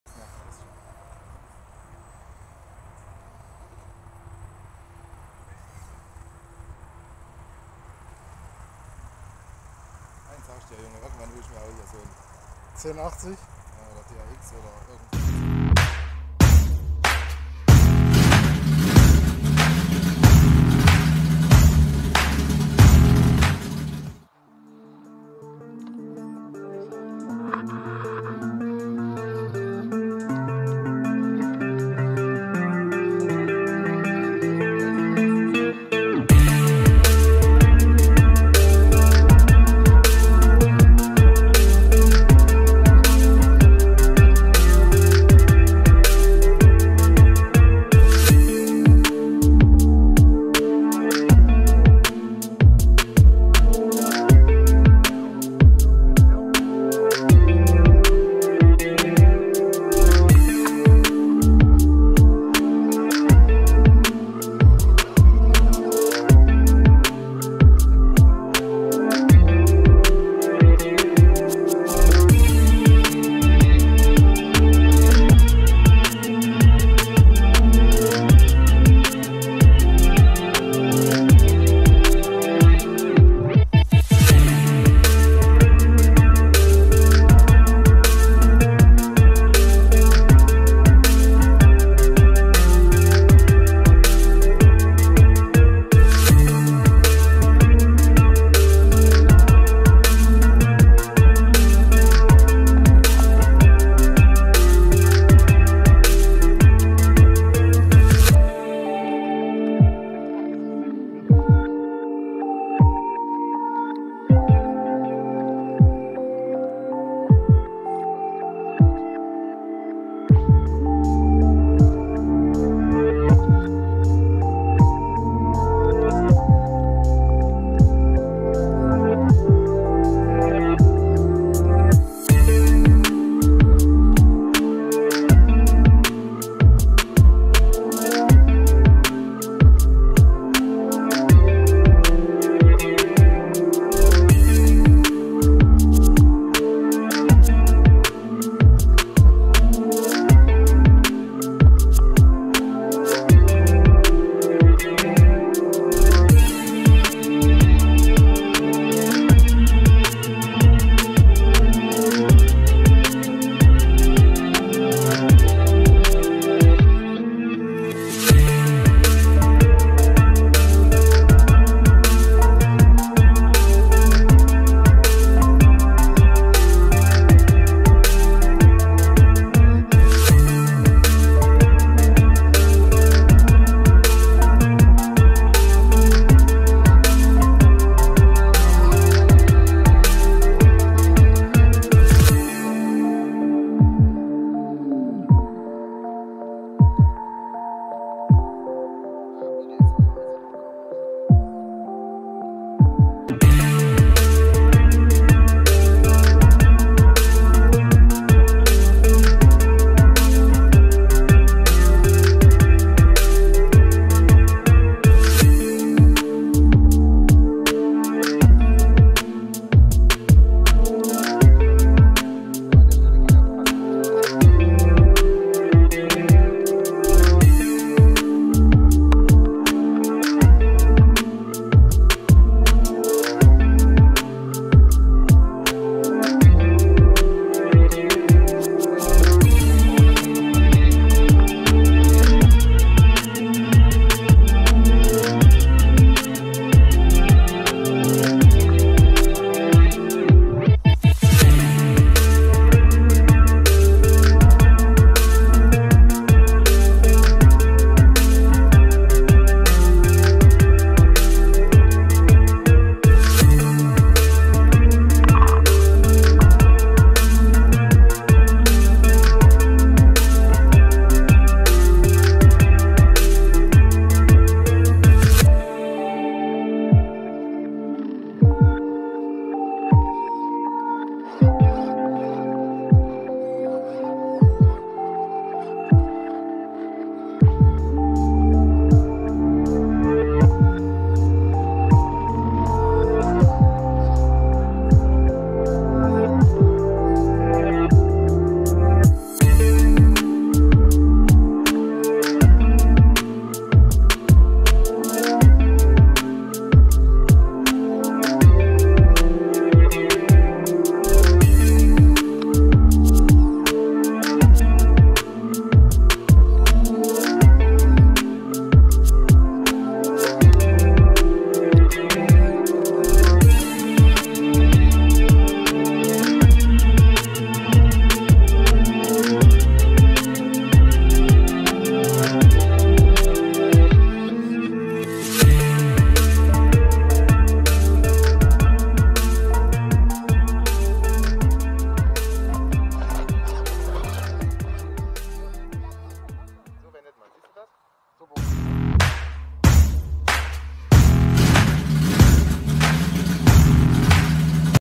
crawlen in Isseroda im Scalepark mit Mica aka @roxorrccar8483 Carson X-Crawlee und MST CFX w 🤩
Wir fahren heute in Isseroda auf der Scaler Strecke des RC Buggy Club Erfurt es sind Fahrzeuge von folgenden Firmen dabei gewesen, und der Mica @roxorrccar8483 ist auch mit dabei.

Carson X-Crawlee
MST CFX w

Schaut auch auf Facebook vorbei unter

Bernd Kretschmer

Oder unter Instagram

Kretschmerbernd2

Euer B.K. RC

Hinterlegt mit freier Musik von Youtube und freier Musik vom gekauften Schneideprogramm.

Link zu unserer Facebook Gruppe

Link zur Facebook Gruppe RCBC